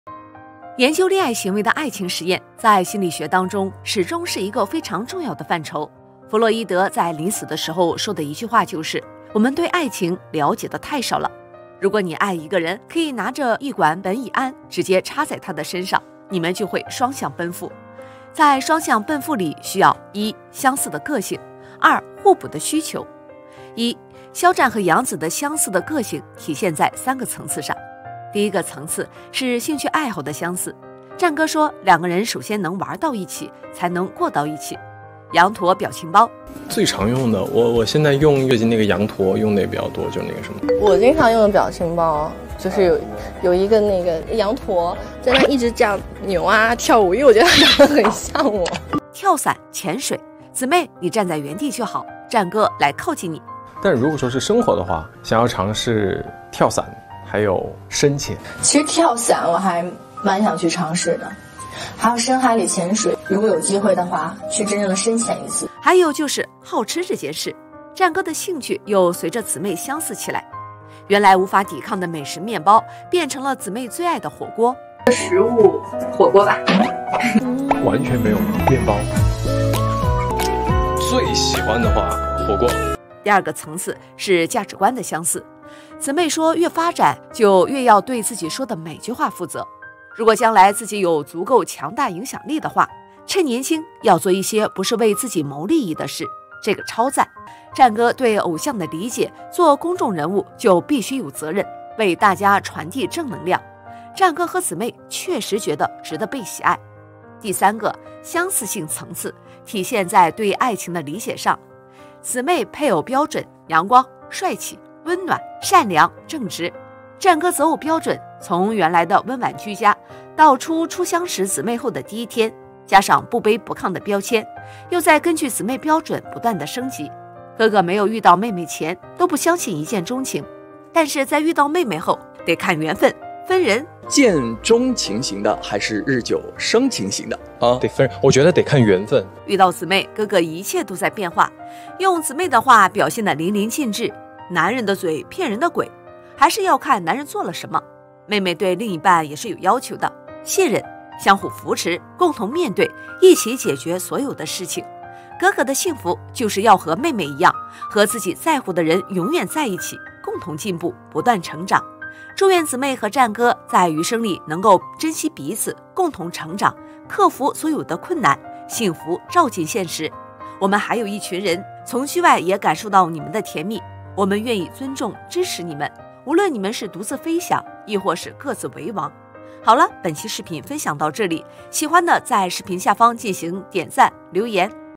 肖戰和楊紫越來越夫妻相？沒有商量都說不過去，不要太好磕了#肖戰#楊紫#夫妻相#肖戰楊紫
如果妳愛一個人，可以拿著一管苯乙胺直接插在他的身上，妳們就會雙嚮奔赴！1.肖戰和楊紫的相似的個性體現在三個層次上：第一個層次，是興趣愛好的相似。戰哥說兩個人首先能玩到一起，才能過到一起！“羊駝”錶情包，跳傘，潛水，紫妹妳站在原地就好，戰哥來靠近妳。還有就是“好吃”這件事……戰哥的興趣又隨著紫妹相似起來。原來無法抵抗的美食面包變成了紫妹最愛的火鍋；第二個層次，是價值觀的相似。紫妹說越發展，就越要對自己說的每句話負責，如果將來自己有足夠強大影響力的話，趁年輕要做一些不是為自己謀利益的事。這個超贊……戰哥對偶像的理解做公眾人物就必須有責任，為大家傳遞正能量。戰哥和紫妹確實覺得值得被喜愛。